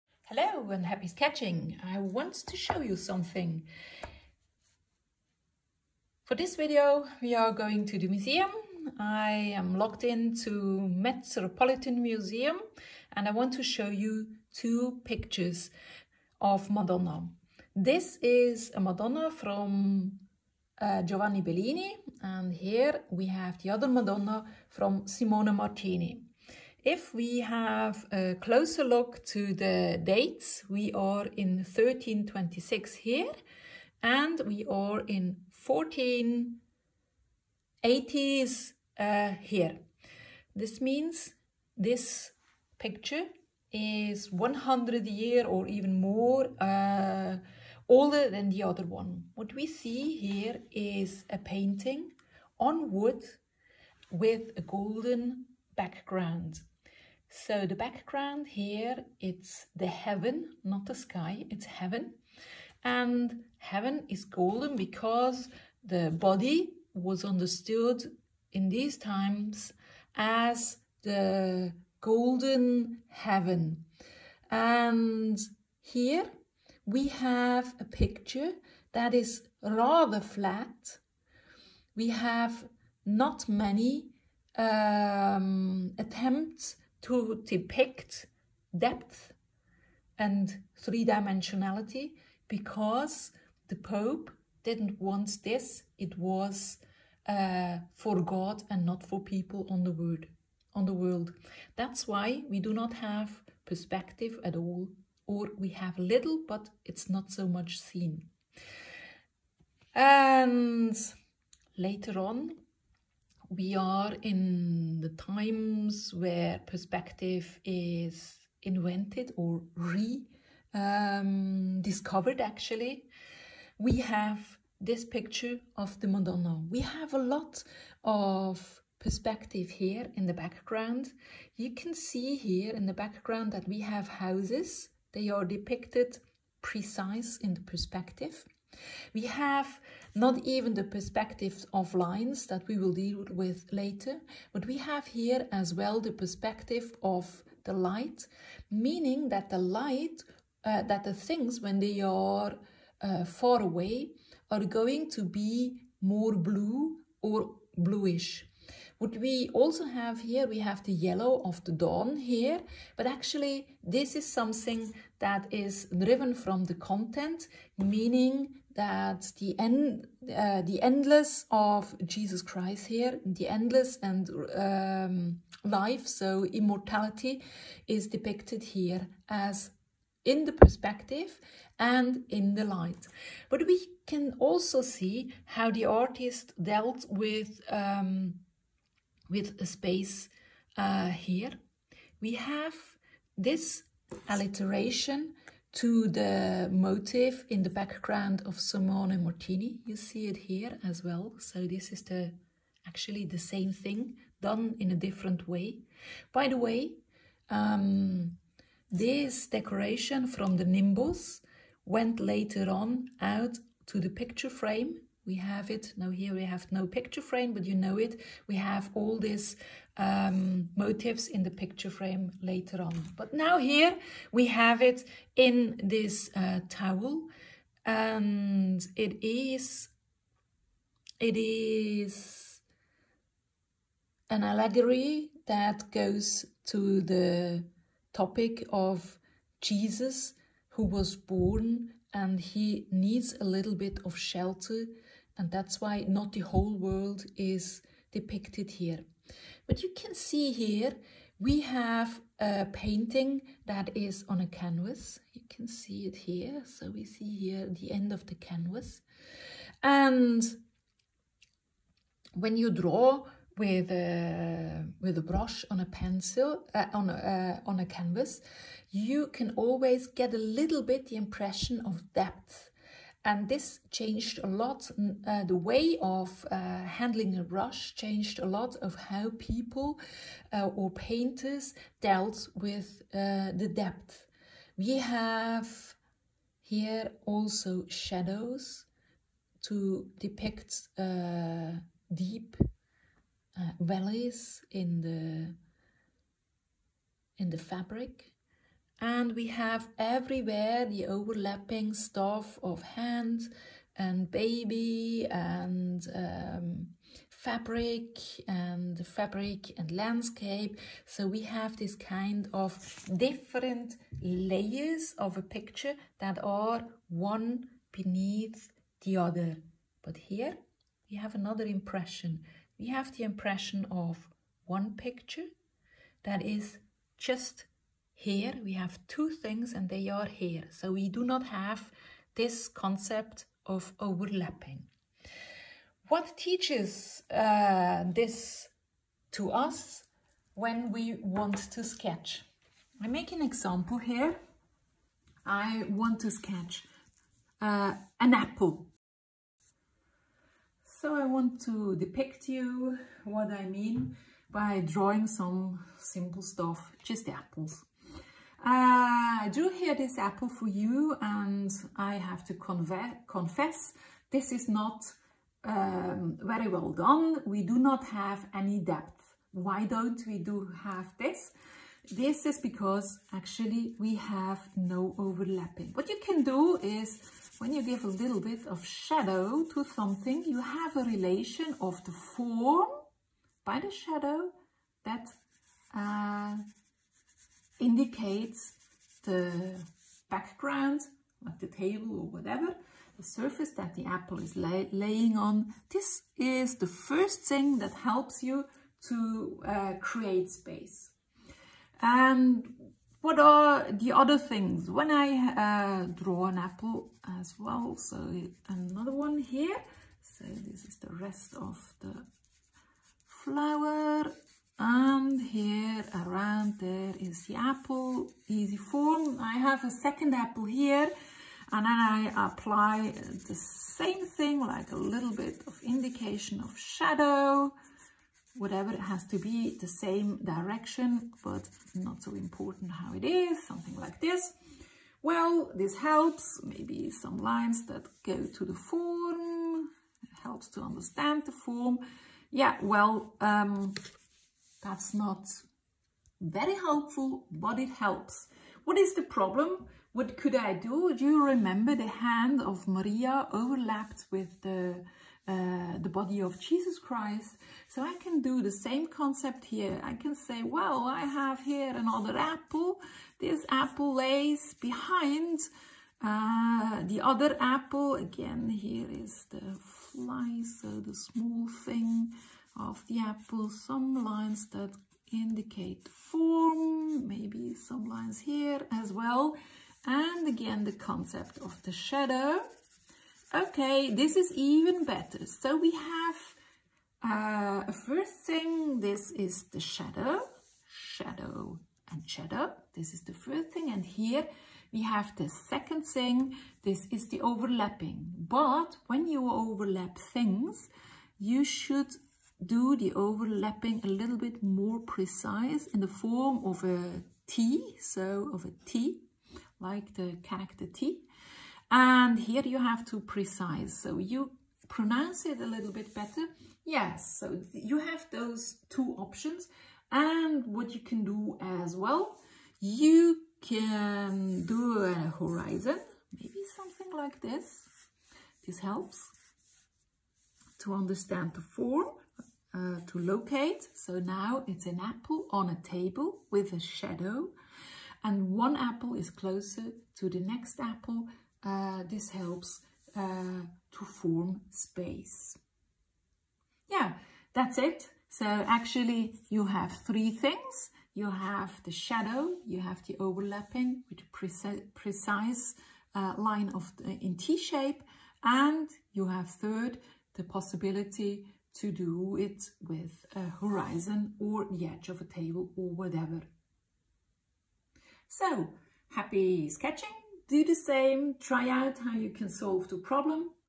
7.4 Apple and Space
Learn to create space with overlapping, shadow and a horizon line.